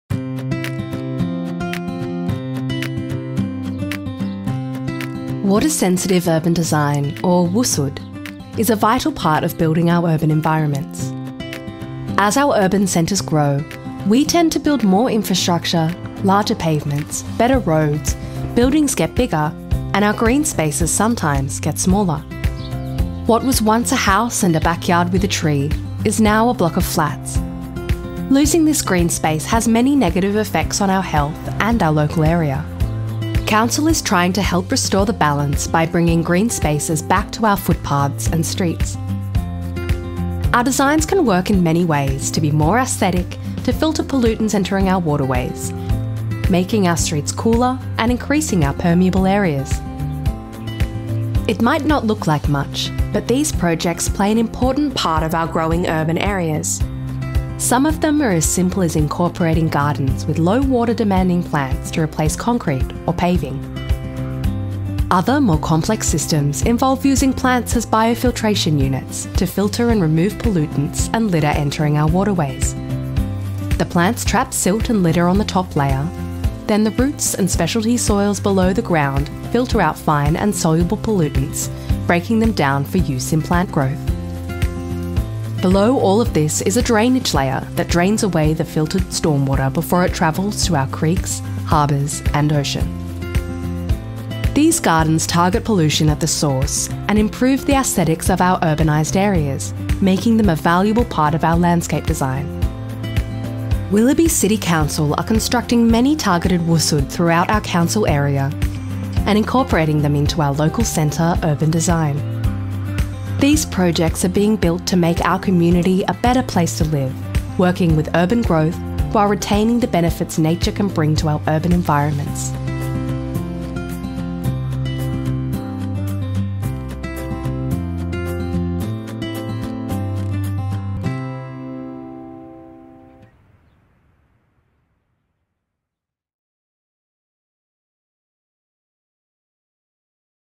Water Sensitive Urban Design
Discover how Water Sensitive Urban Design (WSUD) is helping to bring green spaces back to our urban areas. This video explores how gardens and biofiltration systems improve water quality, reduce pollution, and make our streets cooler and more inviting. Learn how sustainable design is creating a healthier, greener community in Willoughby.